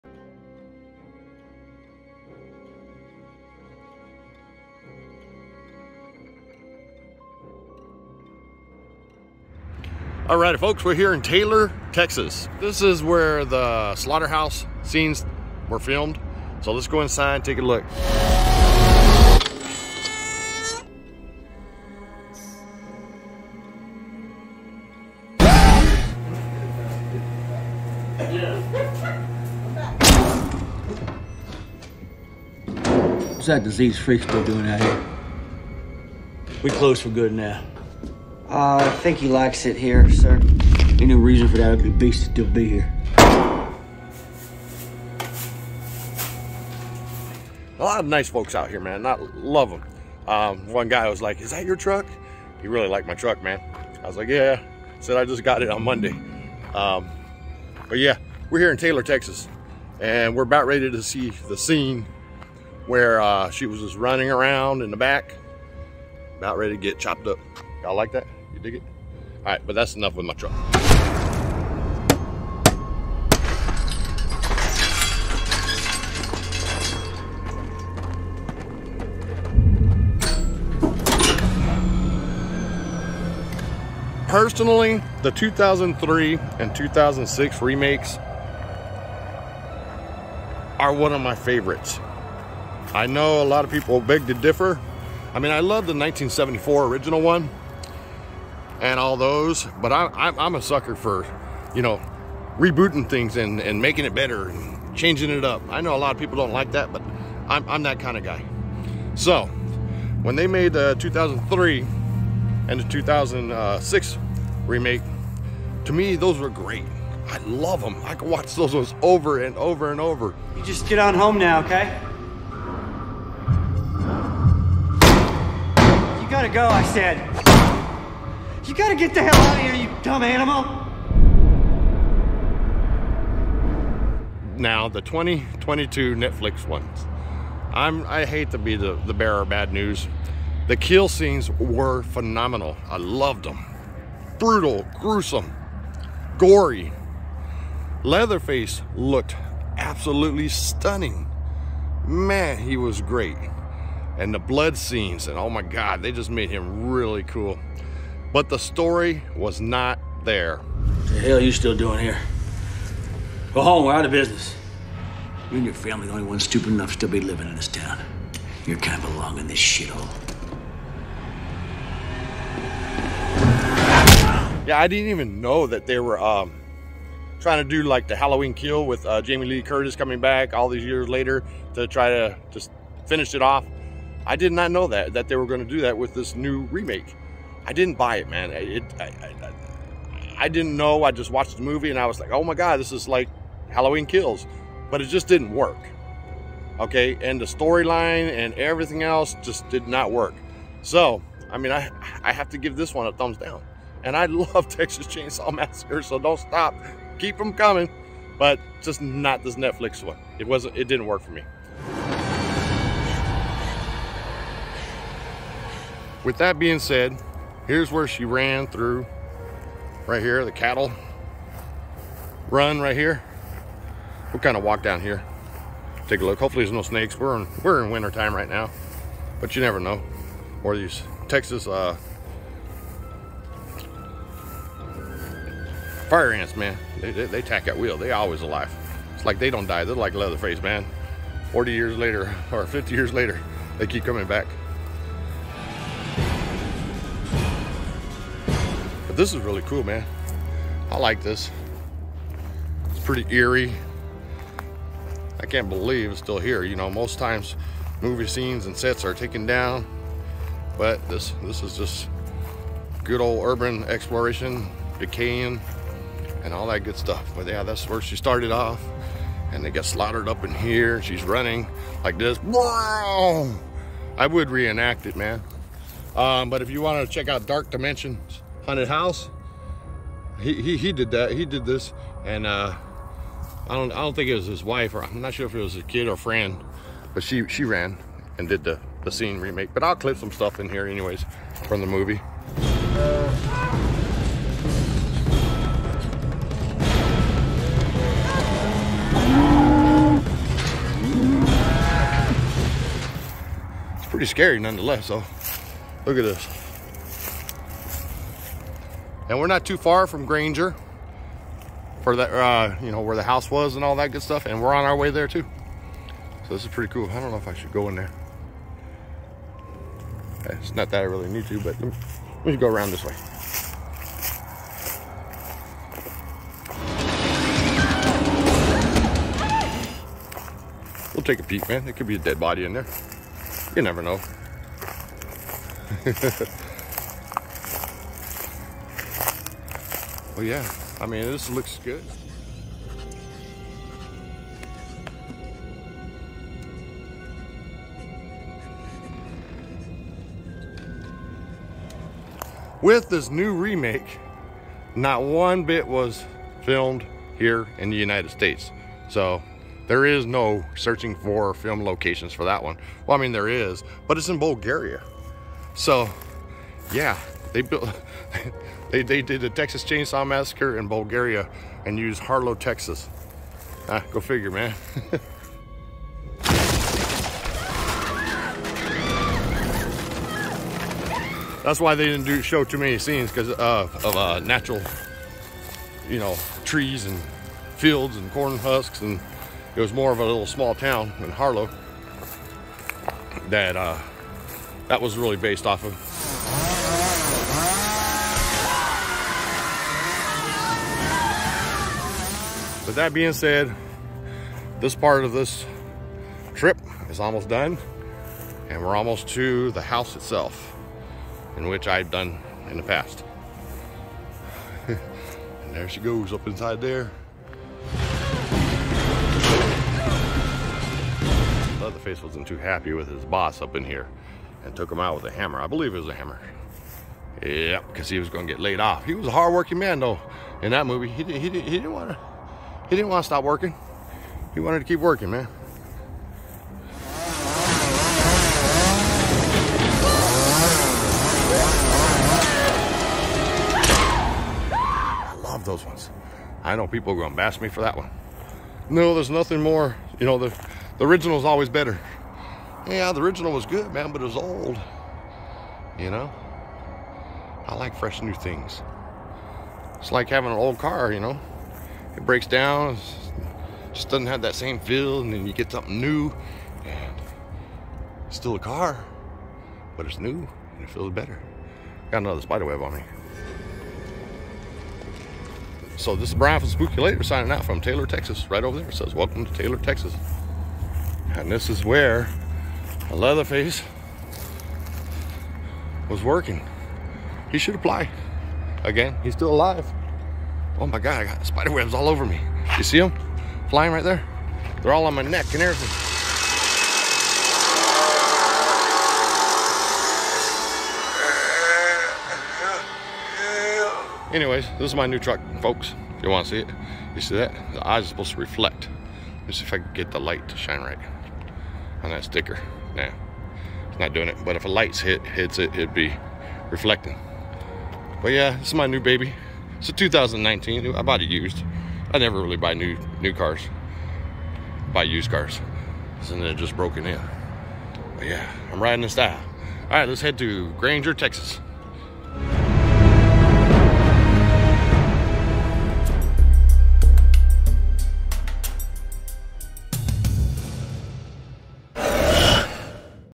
The Slaughterhouse Texas Chainsaw Massacre 2022 filming location!
Today's continued Texas Chainsaw Massacre road trip, Takes our journey to Taylor, Texas up north highway 95. Here we find the Texas Chainsaw Massacre Slaughterhouse Filming Location for the 2003 & 2006 movie remakes with more unsuspecting Leatherface victims. This is stop #2 in our Texas Chainsaw Massacre 2022 filming location round-up. Stop #3 will be the actual Hewitt house used in both the 2003 & 2006 remakes.